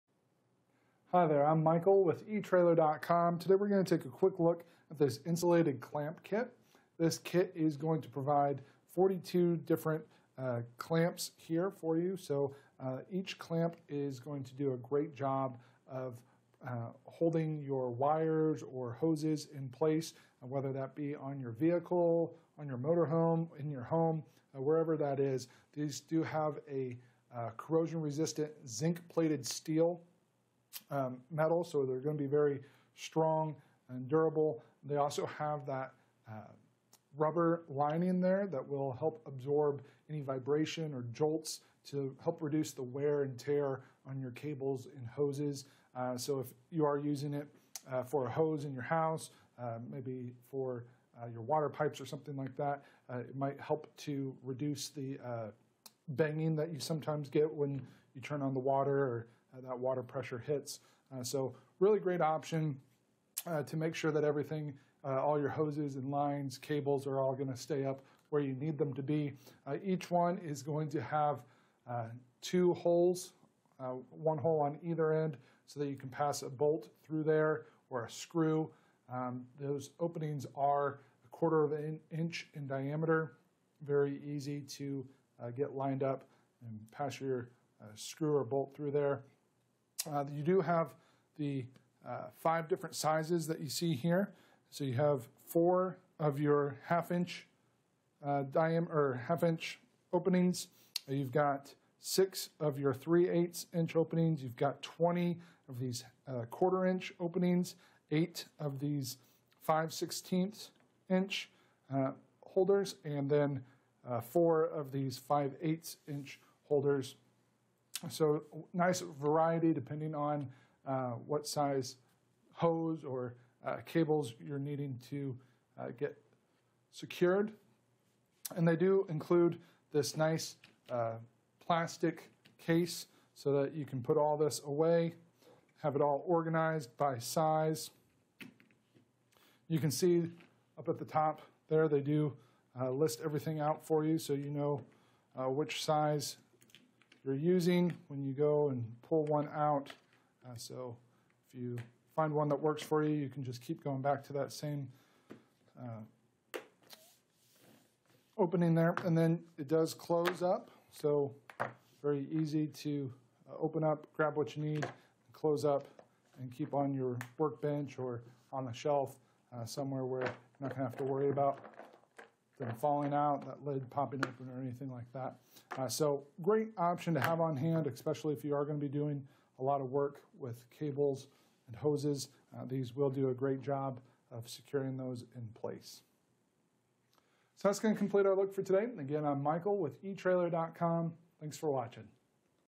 etrailer | Breaking Down the Insulated Clamp Kit
Hi there. I'm Michael with etrailer.com. Today, we're going to take a quick look at this Insulated Clamp Kit. This kit is going to provide 42 different clamps here for you. So, each clamp is going to do a great job of holding your wires or hoses in place. Whether that be on your vehicle, on your motor home, or in your home, or wherever that is.

These do have a corrosion-resistant zinc-plated steel metal. So, they're going to be very strong and durable. They also have that rubber line in there that will help absorb any vibration or jolts to help reduce the wear and tear on your cables and hoses. So, if you are using it for a hose in your house, maybe for your water pipes or something like that, it might help to reduce the banging that you sometimes get when you turn on the water. That water pressure hits.

So, really great option to make sure that everything, all your hoses and lines, cables, are all gonna stay up where you need them to be. Each one is going to have two holes. One hole on either end. So, that you can pass a bolt through there or a screw. Those openings are a quarter of inch in diameter.

Very easy to get lined up and pass your screw or bolt through there. You do have the five different sizes that you see here. So, you have four of your half inch dime or half-inch openings. Then, you've got six of your 3/8 inch openings. You've got 20 of these quarter inch openings.

Eight of these 5/16 inch holders. And then, four of these 5/8 inch holders. So, nice variety depending on what size hose or cables you're needing to get secured. And they do include this nice plastic case, so that you can put all this away, have it all organized by size. You can see up at the top there they do list everything out for you so you know which size you're using, when you go and pull one out. So, if you find one that works for you, you can just keep going back to that same opening there and then it does close up. So, very easy to open up, grab what you need, close up. And keep on your work bench or on a shelf. Somewhere where you're not gonna have to worry about them falling out that led popping up on or anything like that. So, great option to have on hand, especially if you are going to be doing a lot of work with cables and hoses, these will do a great job of securing those in place. So, that's going to complete our work for today.